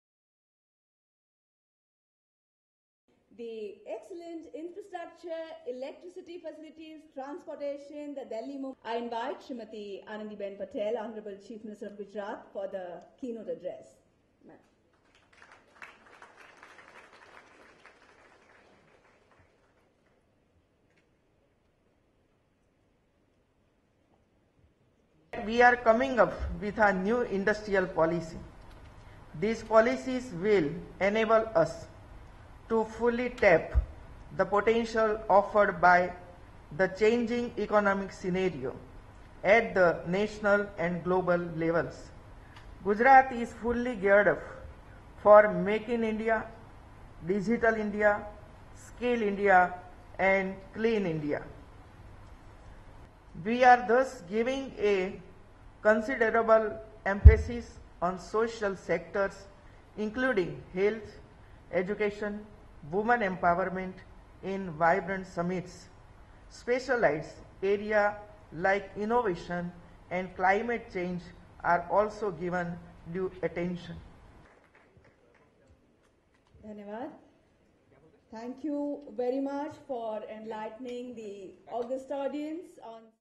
Western Indian state to tap changing economic scenario with new industrial policies
Addressing a news conference the chief minister says that the western Indian state of Gujarat is geared to tap the changing economic scenario with new industrial policies that would put the nation at the global forefront.